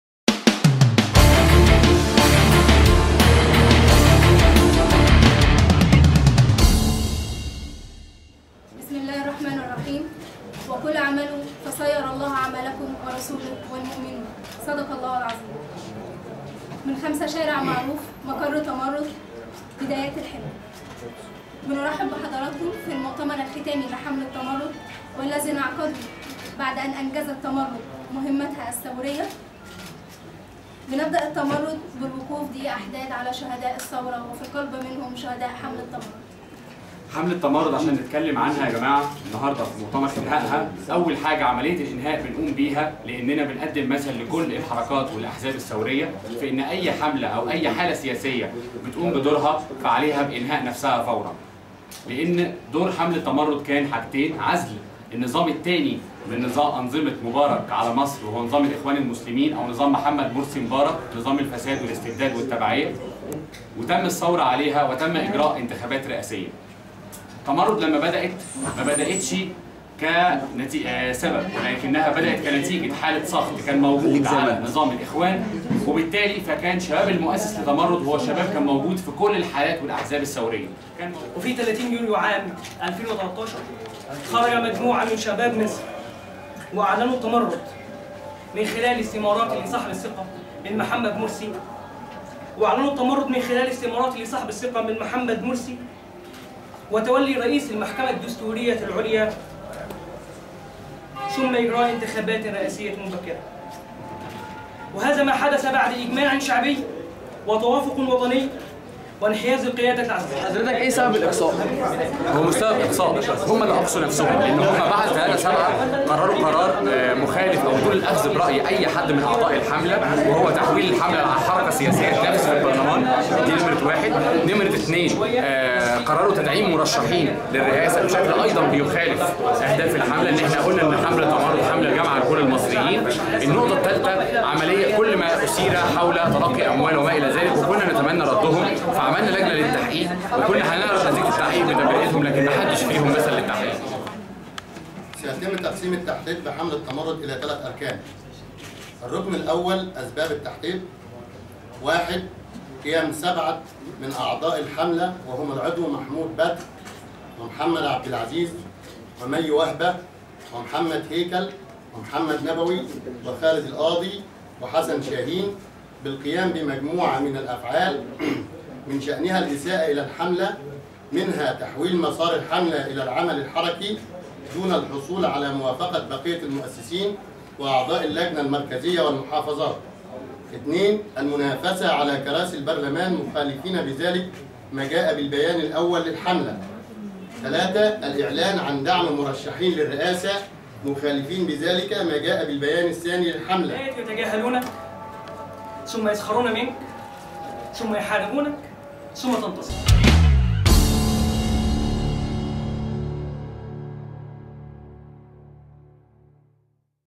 "تمرد" تعلن انتهاء عملها.. وتؤكد: تم تدشنها لمحاربة "مرسي"
تقرير: محمد أشرف

جريدة الوطن
جريدة يومية مستقلة